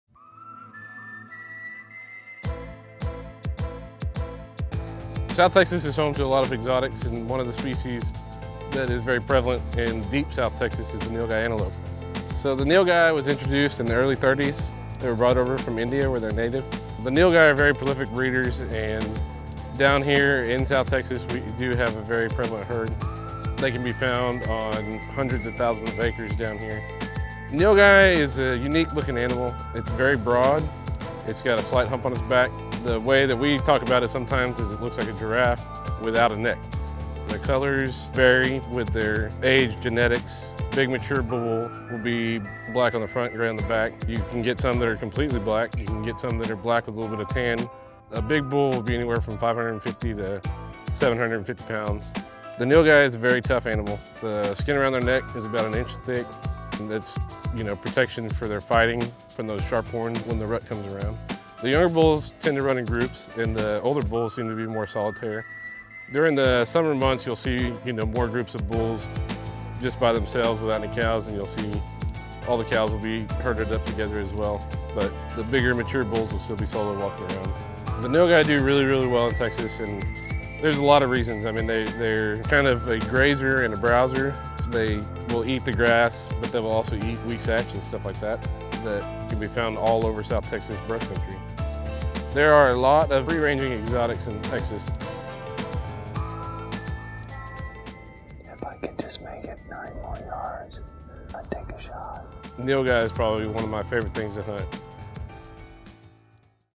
The Ford Outfitters - What's a Nilgai?
Outfitter Adam Batot of Double Shot Outfitters talks about the Texas Nilgai and how prevalent they are to Texas.

Enter for a chance to win a 2017 Ford Super Duty and a trip to the legendary King Ranch in Texas!